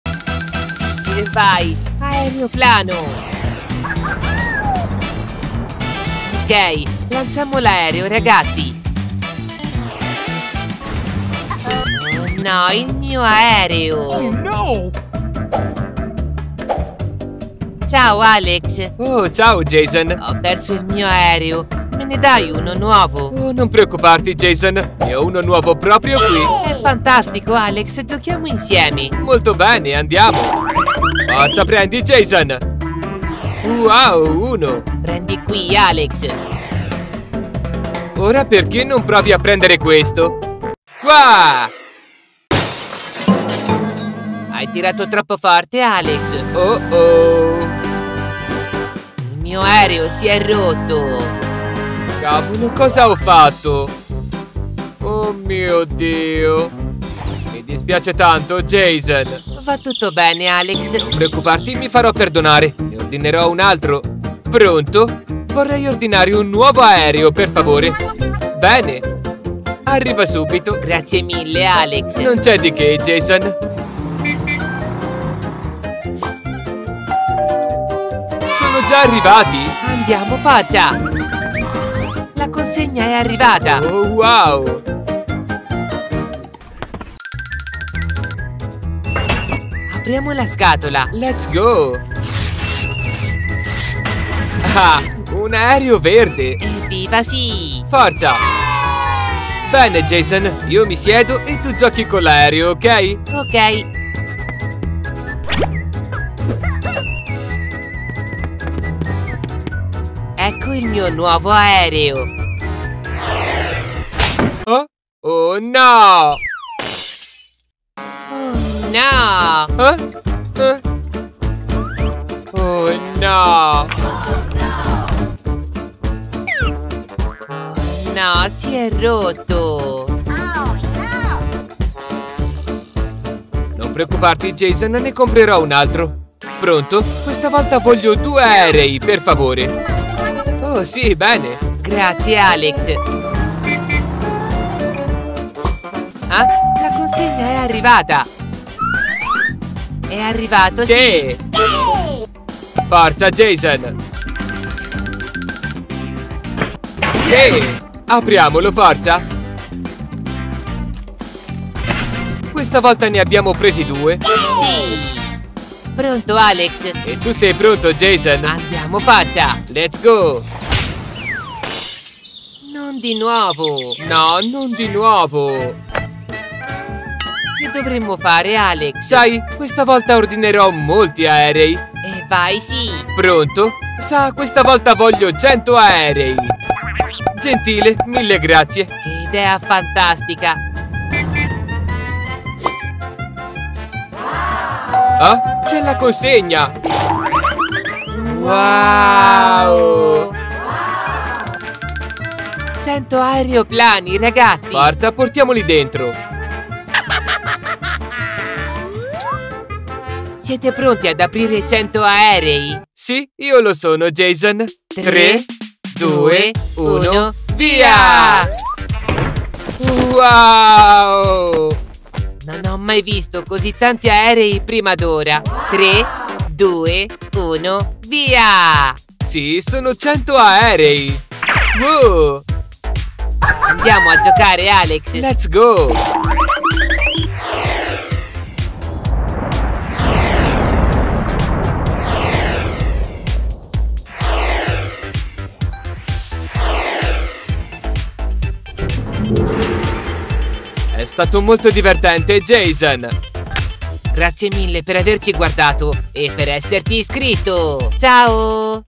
Jason e Alex giocano con 100 aeroplani giocattolo! Giochi all'aperto per bambini
Jason e Alex giocano con gli aeroplanini giocattolo all'aperto. Ma ogni volta succedono problemi: gli aerei si guastano. Jason e Alex hanno ordinato 100 aeroplanini giocattolo con cui giocare tutto il giorno. Giochi all'aperto per bambini!